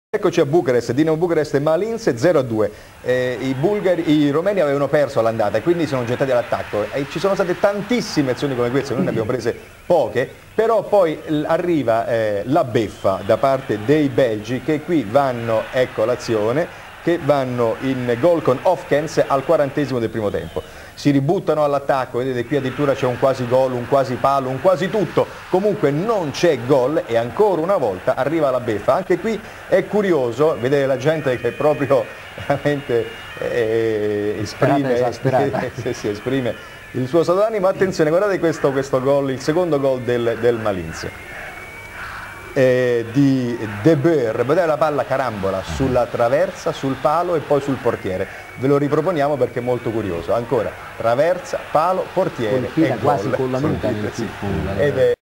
Dinamo Bucuresti - FC Malines (KV Mechelen) 0-2 (30.09.1987, Cup Winners Cup, turul I, meci retur)
un alt meci trist, multe ratari si 2 goluri ciudate primite, cel de-al doilea este antologic
Malines avea sa castige trofeul in acel an, invingand Ajax-ul in finala si chiar si Supercupa Europei, invingand pe PSV Eindhoven.
In 2002 echipa a intrat in faliment si a fost retrogadata in divizia a 3-a, revenind in prima divizie in 2007.

goluri: Wim Hofkens 39', Piet den Boer 71'

Dinamo
 Dumitru Moraru
 Ioan Varga
 Mircea Rednic
 Alexandru Nicolae
 Iulian Mihăescu
 Dănuț Lupu
 Ioan Lupescu
 Costel Orac (George Timiş 61')
 Dorin Mateuţ
 Marian Damaschin (Ilie Balaci 46'
 Rodion Cămătaru
Antrenor: Mircea Lucescu

Malines
 Michel Preud'homme
 Graeme Rutjes
 Wim Hofkens
 Lei Clijsters
 Paul Theunis (Raymond Jaspers 34')
 Koen Sanders
 Erwin Koeman
 Pascal De Wilde
 Bert Cluytens
 Eli Ohana (Joachim Benfeld 70')
 Piet den Boer
Antrenor:  Aad de Mos
Arbitru: Pietro D'Elia (Italia)